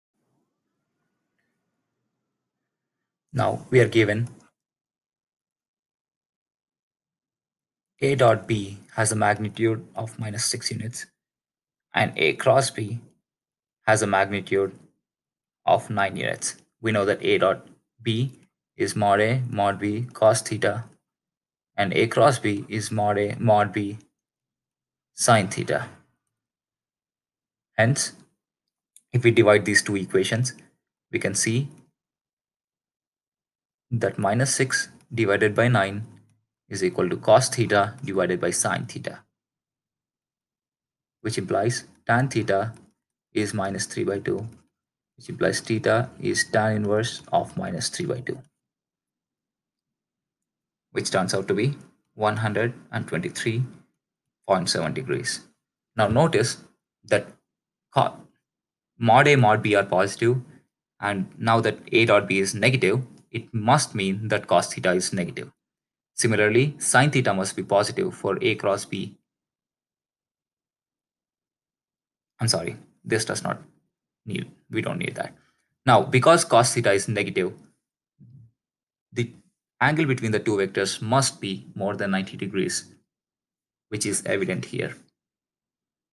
Vectors $overrightarrow{A}$ and $overrightarrow{B}$ have scalar product $ $6 00, and their vector pr
Vectors
and
have scalar product
6.00, and their vector product has magnitude
9.00. What is the angle between these two vectors?